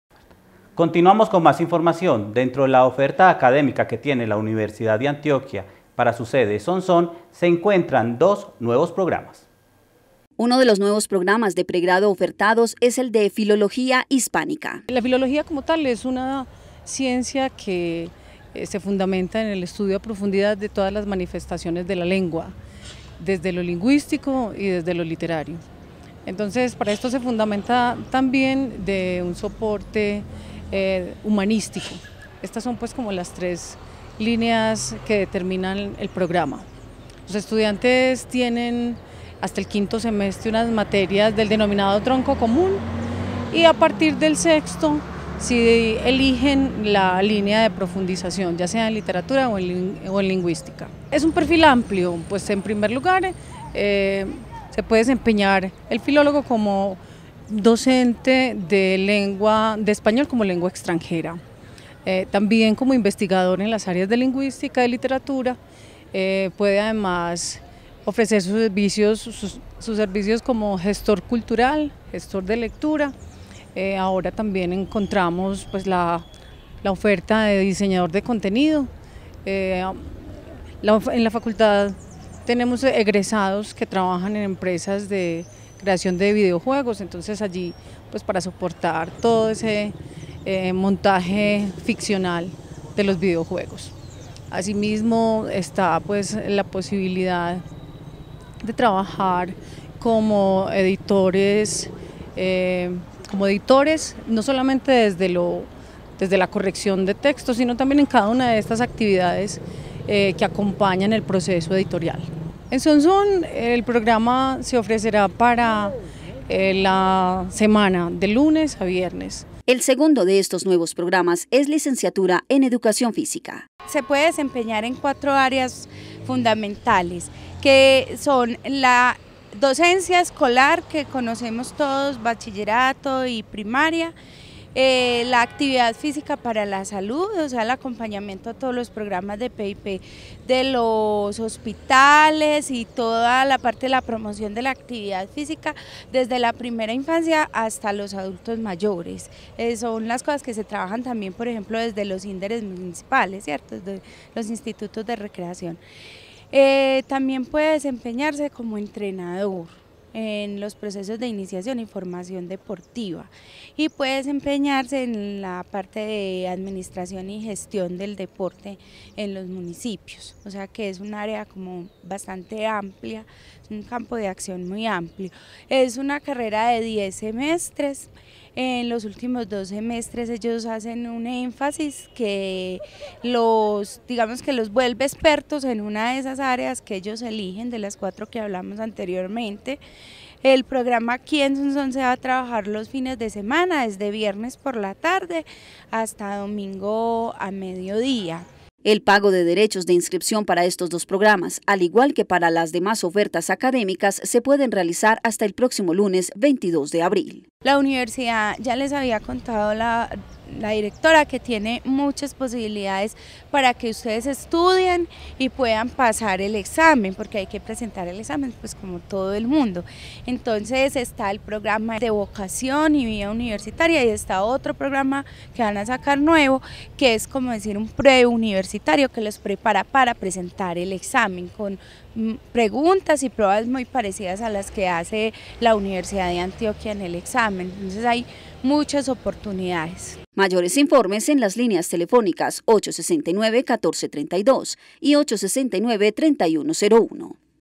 DOS NUEVOS PROGRAMAS OFERTA LA UDEA EN SONSÓN
Dentro de la oferta académica que tiene la Universidad de Antioquía para su sede Sonsón se encuentran dos nuevos programas.